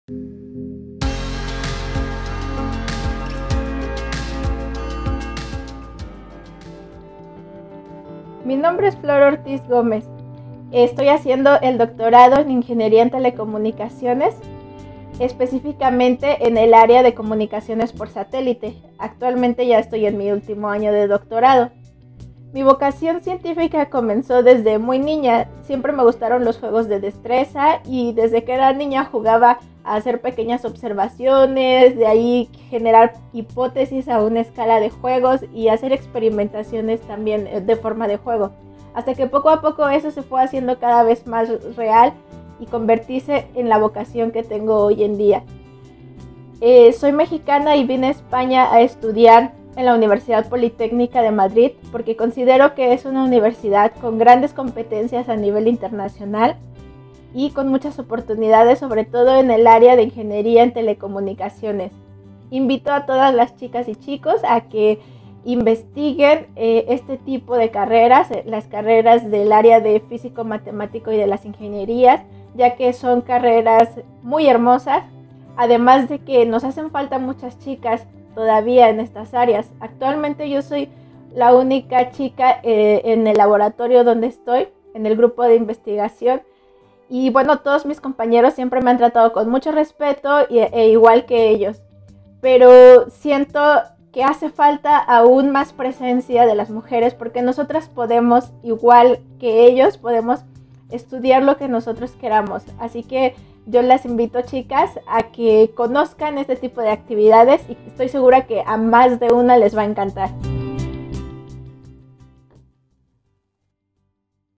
DÍA INTERNACIONAL DE LA MUJER Y LA NIÑA EN LA CIENCIA - Flor Ortiz
Con motivo del Día Internacional de la Mujer y la Niña en la Ciencia, estudiantes de doctorado de la UPM animan a las jóvenes a apostar por su vocación científica.